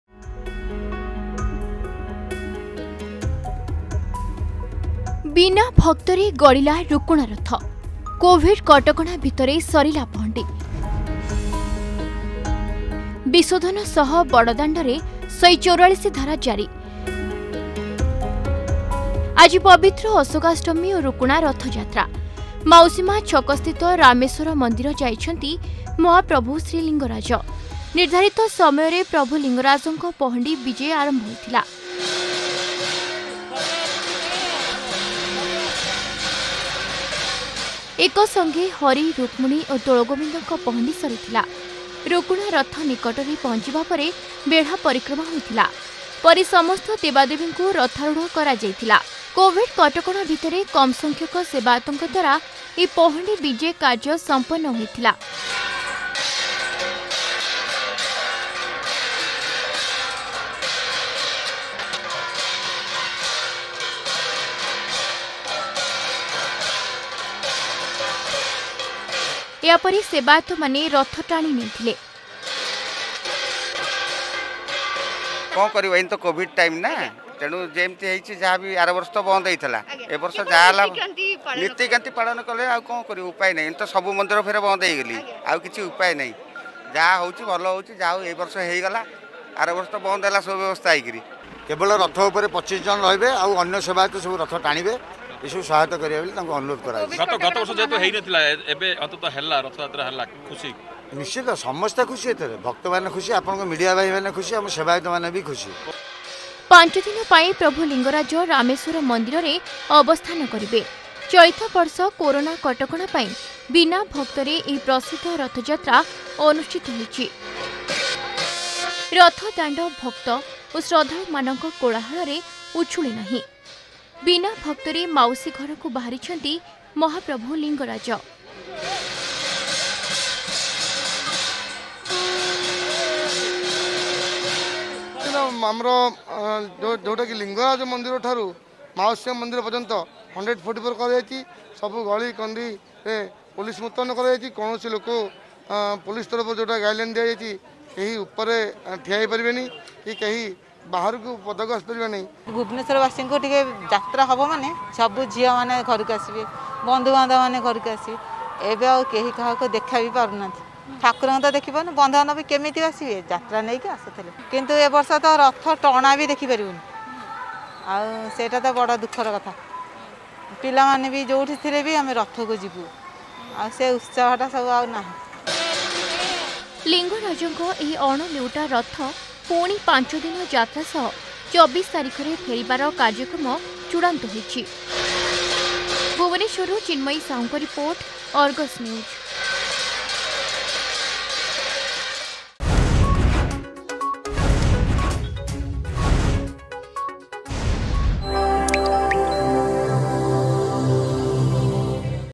ବିନା ଭକ୍ତରେ ଗଡିଲା ରୁକୁଣା ରଥ
ଗଡିଲା ଅଣଲେଉଟା ରୁକୁଣା ରଥ । ବିନା ଭକ୍ତରେ ମାଉସୀ ମାଆ ଛକ ସ୍ଥିତ ରାମେଶ୍ୱର ମନ୍ଦିର ଗଲେ ମହାପ୍ରଭୁ ଶ୍ରୀଲିଙ୍ଗରାଜ । ଏଥିପାଇଁ ମନ୍ଦିର ସମ୍ମୁଖରେ 144 ଧାରା ଜାରି ହୋଇଥିବା ବେଳେ ଚଳିତ ବର୍ଷ କରୋନା ପାଇଁ ବିନା ଭକ୍ତରେ ଗଡିଛି ରଥ । ସେବାୟତ ଓ ପୋଲିସ ମାନେ ରଥ ଟାଣି ନେଇଛନ୍ତି.